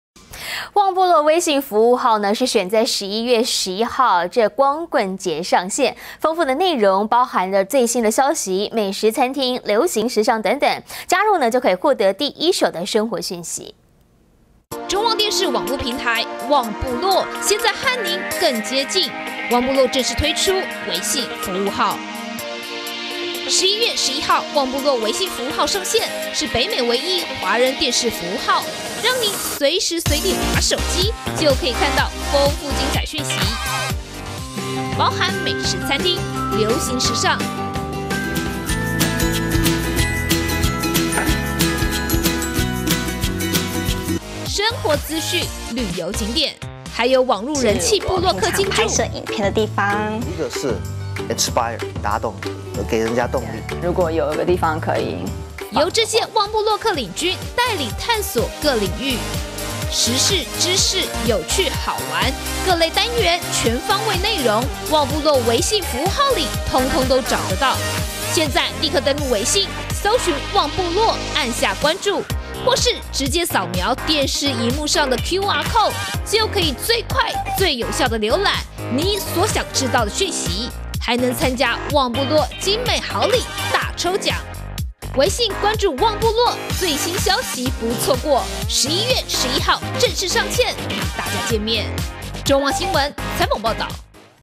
2016.11.10中旺新聞-2 「旺部落」微信服務號 11/11正式上線
千呼萬喚始出來！旺部落微信服務號選在11/11上線，豐富的內容包括最新消息、美食餐廳、流行時尚等等，加入即可獲得第一手生活訊息。